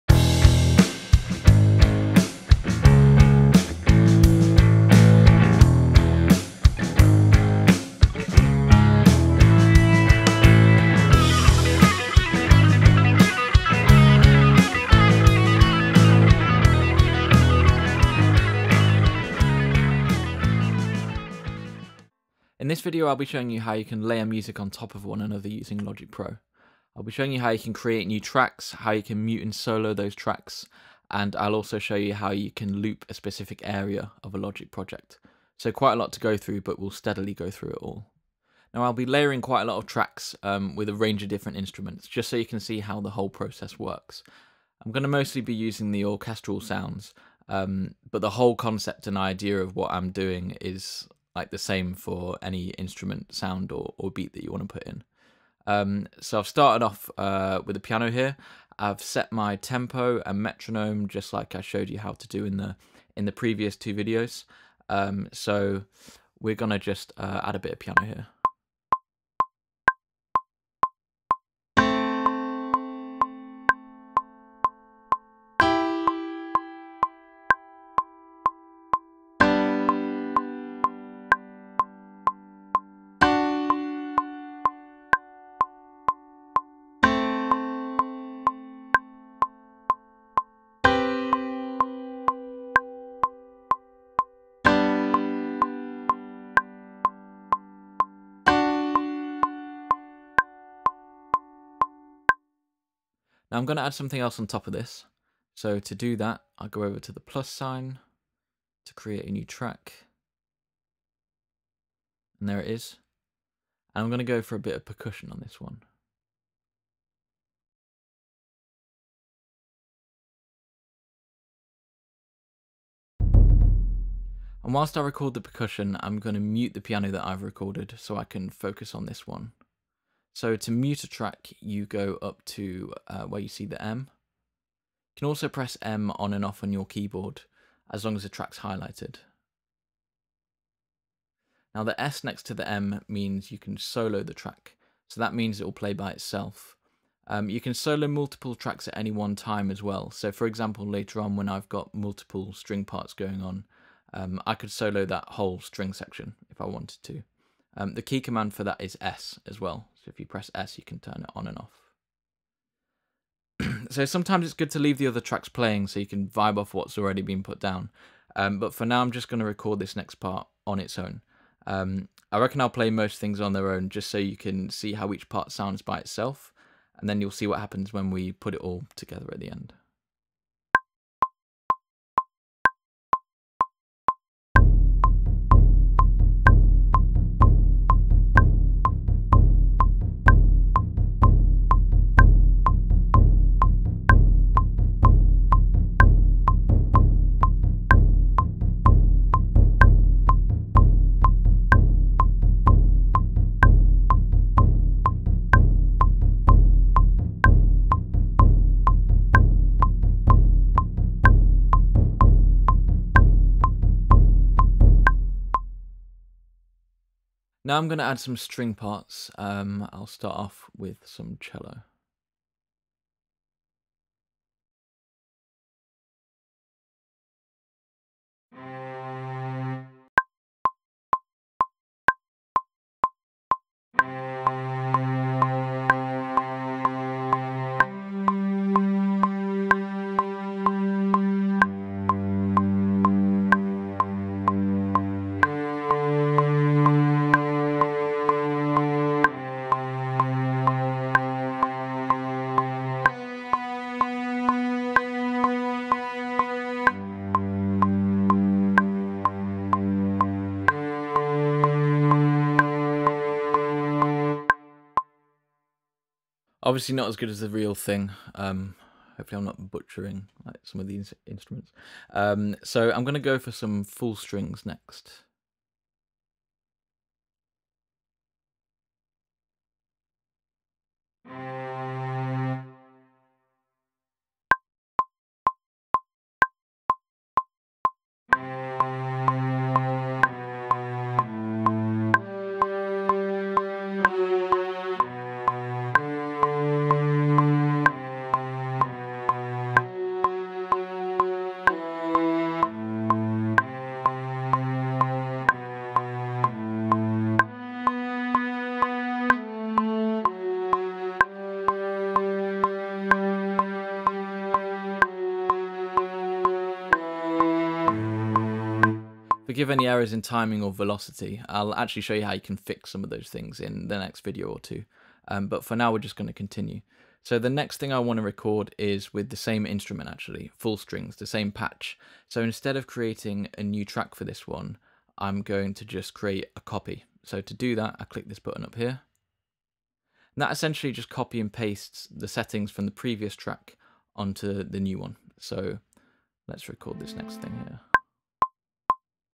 Layering tracks/sounds (MIDI) - Part 1 - Logic Pro X tutorial
In this video, we begin creating a short track from scratch using sounds from the MIDI sound library. Time stamps for specific topics are below:

0:38 (throughout). Construct a track using MIDI.
1:43 Layer tracks.
2:04 Mute tracks.
2:21 Solo tracks.
5:26 Duplicate tracks.